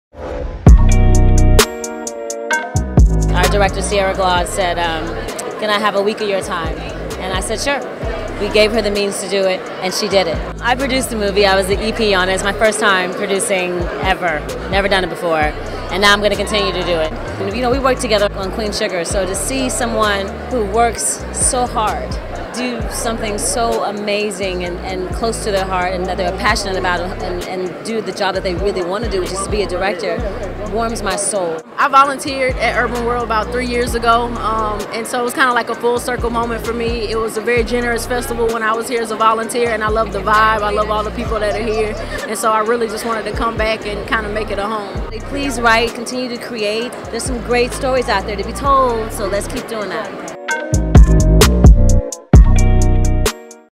RUTINA WESLEY
Producer and Actress Rutina Wesley at the premiere of Last Looks at the 21st Annual Urbanworld Film Festival.